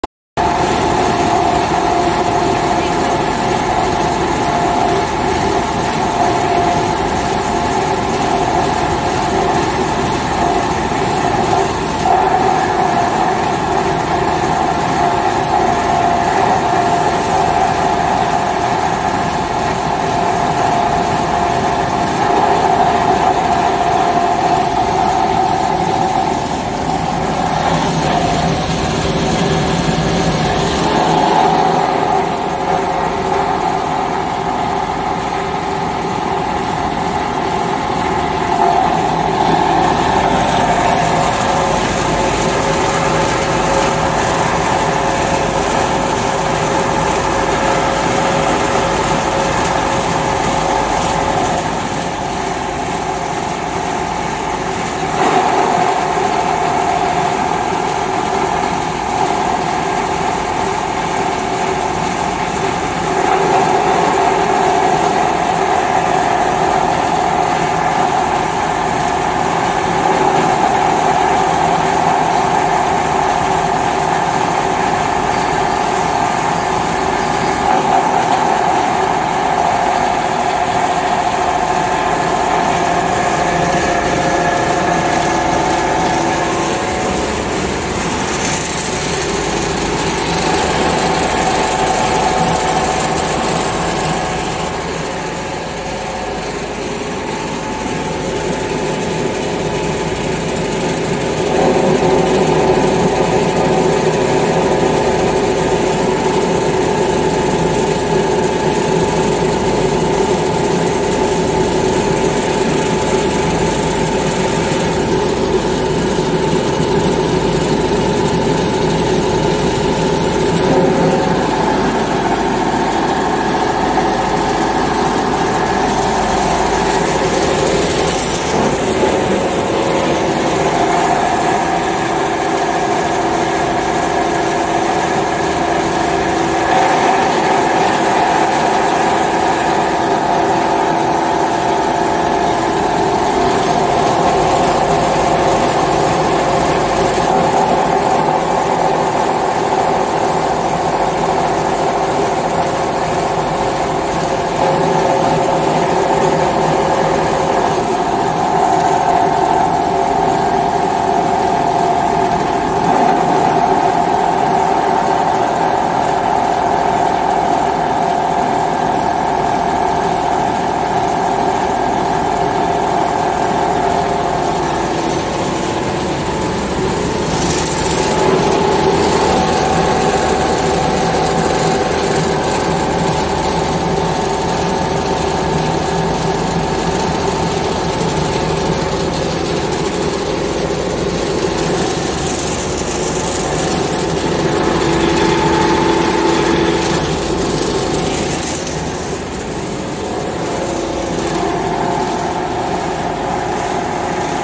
Mini Copper Wire Recycling Machine Separates Wires Cleanly And Efficiently
The new mini copper wire recycling machine separates wires cleanly and efficiently can quickly and efficiently recycle waste wires and obtain pure copper, which is cost-effective.